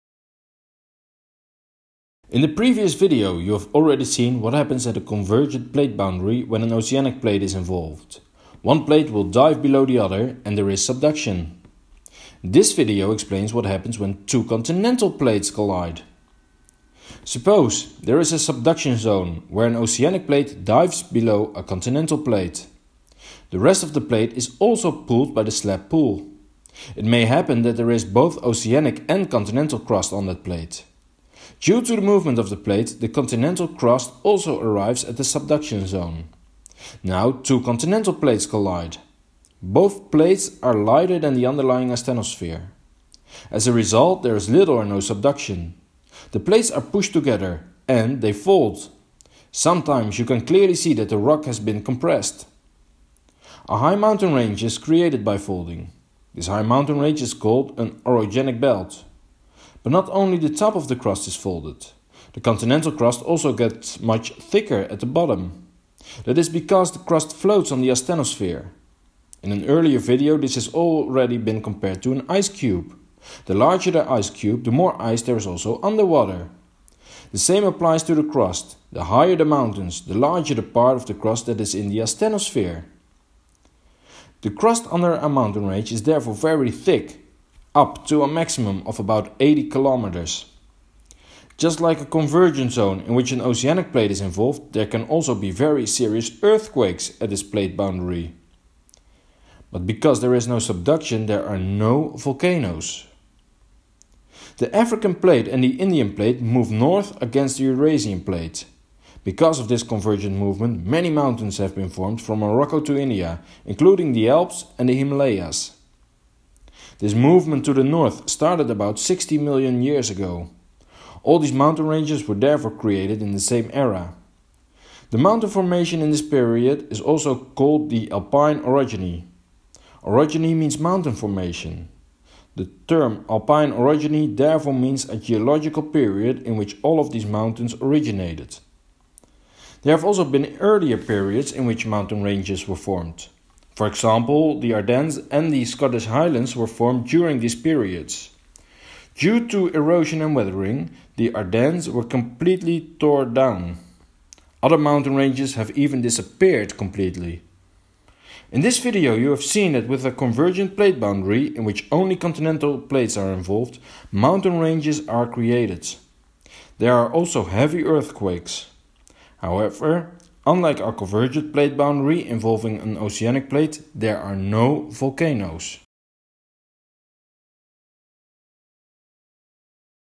How mountain ranges are formed at convergent plate boundaries
In the previous video you have already seen what happens at a convergent plate boundary when an oceanic plate is involved; one plate will dive below the other and there is subduction. This video explains what happens when two continental plates collide.

Suppose; there is a subduction zone where an oceanic plate dives below a continental plate. The rest of the plate is also pulled by the slab pull. It may happen that there is both oceanic and continental crust on that plate. Due to the movement of the plate, the continental crust also arrives at the subduction zone. Now two continental plates collide. Both plates are lighter than the underlying asthenosphere. As a result, there is little or no subduction. The plates are pushed together and fold. Sometimes you can clearly see that the rock has been compressed .

A high mountain range is created by folding. This high mountain range is called an orogenic belt.
But not only the top of the crust is folded. The continental crust also gets much thicker at the bottom. That is because the crust floats on the asthenosphere. In an earlier video this has already been compared to an ice cube. The larger the ice cube, the more ice there is also under water. The same applies to the crust: the higher the mountains, the larger the part of the crust that is in the asthenosphere. The crust under a mountain range is therefore very thick, up to a maximum of about 80 kilometers!

Just like a convergent zone in which an oceanic plate is involved, there can also be very serious earthquakes at this plate boundary. But because there is no subduction, there are no volcanoes!

The African plate and the Indian plate move north against the Eurasian plate. Because of this convergent movement, many mountains have been formed from Morocco to India, including the Alps and the Himalayas.

This movement to the north started about 60 million years ago. All these mountain ranges were therefore created in the same era. The mountain formation in this period is also called the alpine orogeny. Orogeny means mountain formation. The term alpine orogeny therefore means a geological period in which all of these mountains originated.

There have also been earlier periods in which mountain ranges were formed. For example, the Ardennes and the Scottish Highlands were formed during these periods. Due to erosion and weathering, the Ardennes were partly tore down. Other mountain ranges have even disappeared completely!

In this video you have seen that with a convergent plate boundary in which only continental plates are involved, mountain ranges are created. There are also heavy earthquakes. However, unlike a convergent plate boundary involving an oceanic plate, there are no volcanoes.